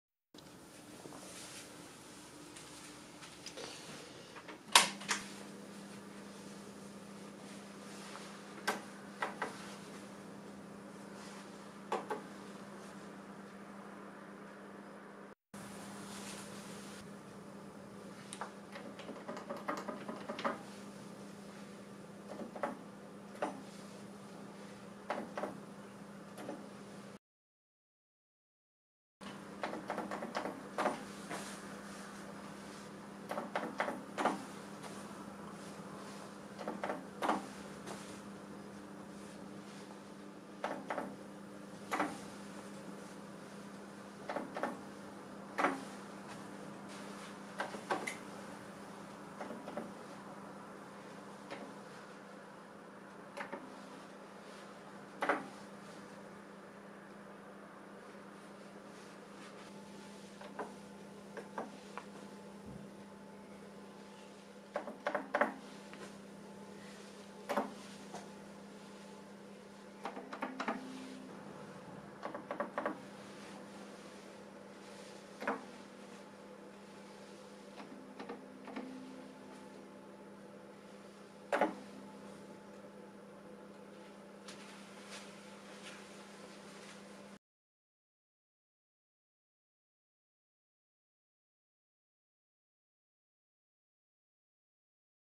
Canon 130s
The Canon 130s is a very early electronic calculator (late 60's).  It's logic is build of transistors and diodes.
After some repairs my copy is gradually allowing some calculations.
Addition and subtraction are possible.  Division works but results are not reliable for all digits.  Multiplication doesn't work yet.
After many repair attempts and replacement of several suspicious components a full demo is possible.